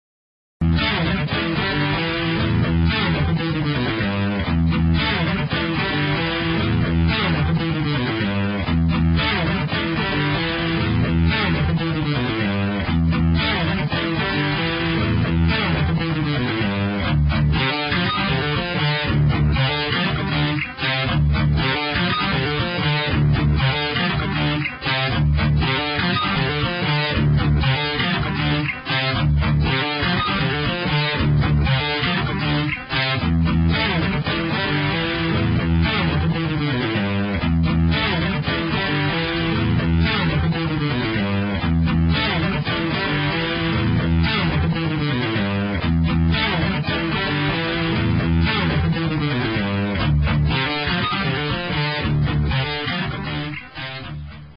LED Guitar Ring Demo
Read The Discription:

This is laser rings, so they can be played on guitars, or just in the dark waving your hands around.

There are many other laser products sold at